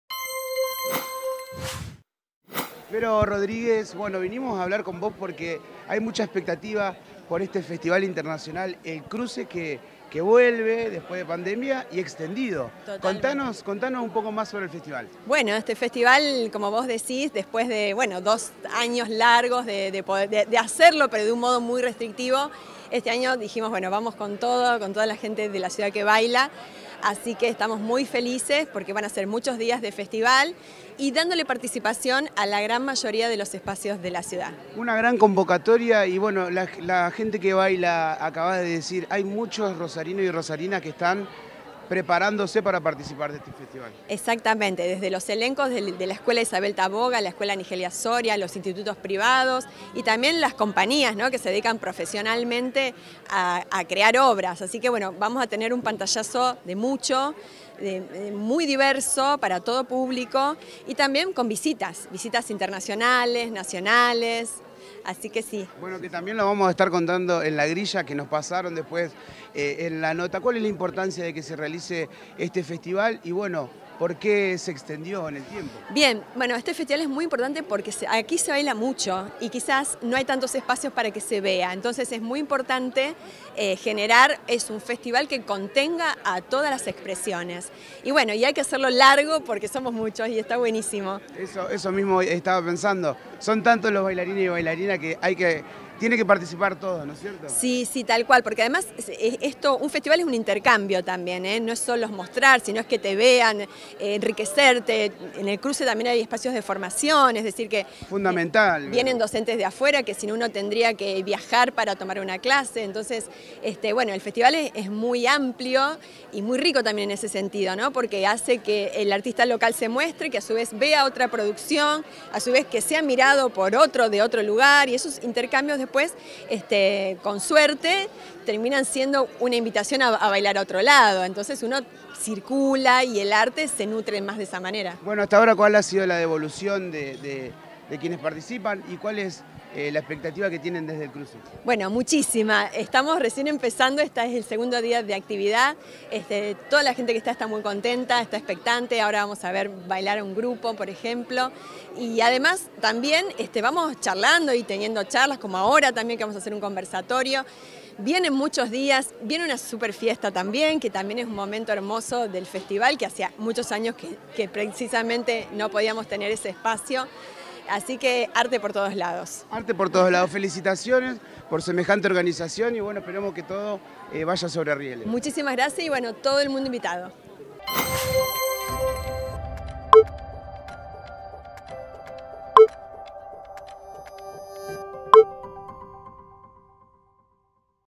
El festival internacional de artes escénicas contemporáneas El Cruce comenzó a puro baile en Rosario
Con mucha expectativa y con muchas ganas comenzó un evento muy esperado por bailarines y bailarinas de la ciudad que se extenderá hasta el 12 de noviembre con una grilla muy completa y variada.